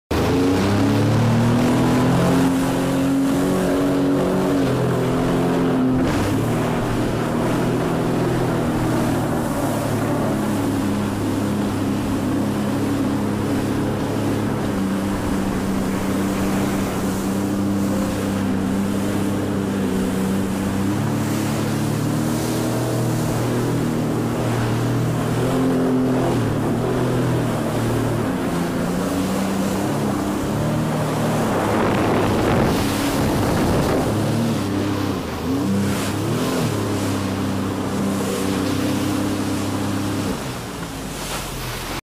jumping some waves in ocean in front of Cupsogue Beach
Yea yea yea!!! I try and get the most out of the waves on the new Doo. These vids were shot on Todd's Bold that he, of course, has already gotten rid of and got another phone! Look how nice that ocean looks! We truly live in Paradise.